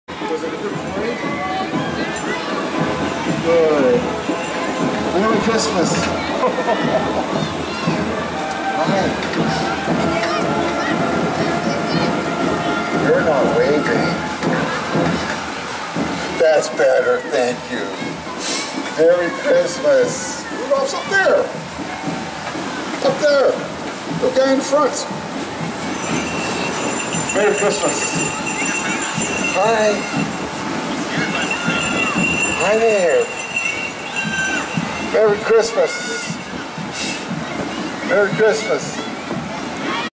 Santa Claus on the Lake Shore
Santa Claus travelling along Lake Shore Blvd. W. in New Toronto as he brings up the end of the Etobicoke Lakeshore Santa Claus Parade.